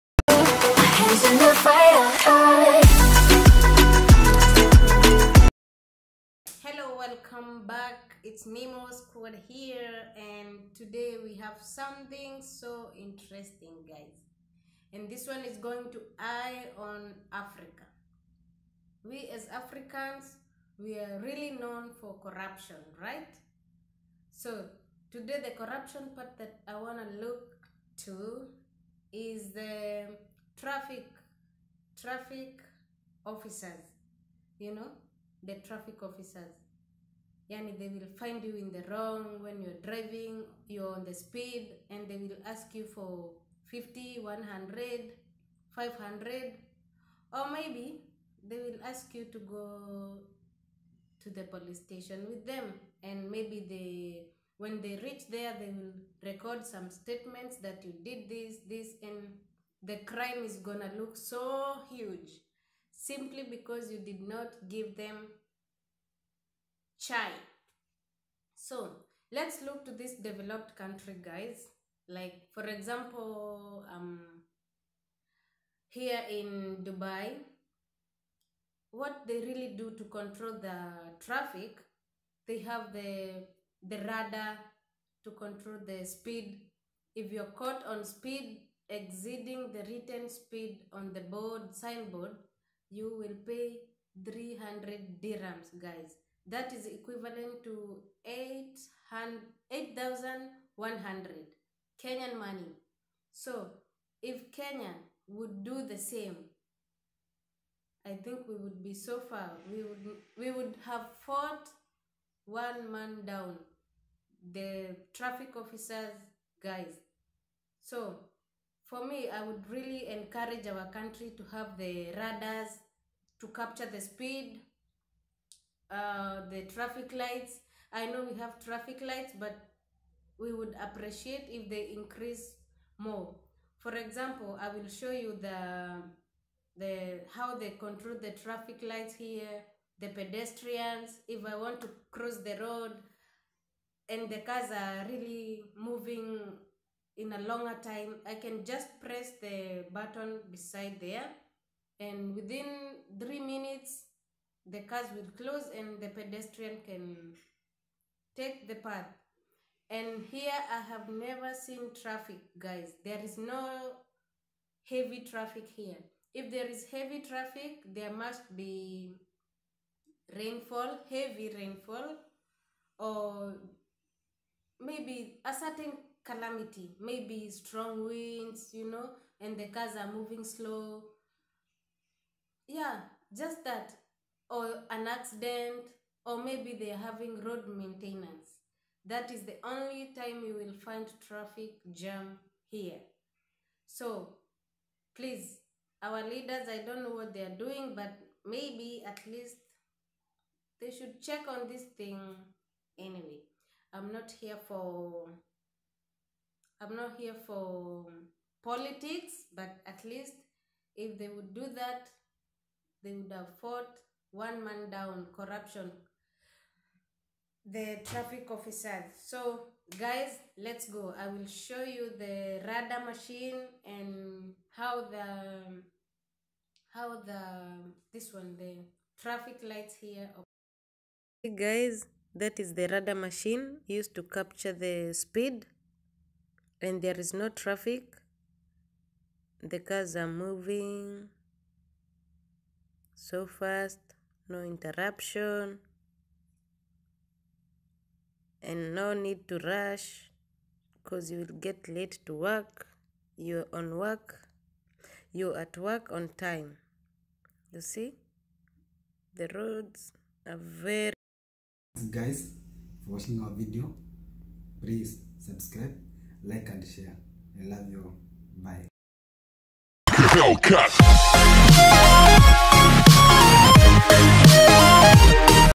How to eliminate corrupt traffic officer
Simple measures on road users,we can avoid officers and drivers fights over corruption issues by moving to the digital ways.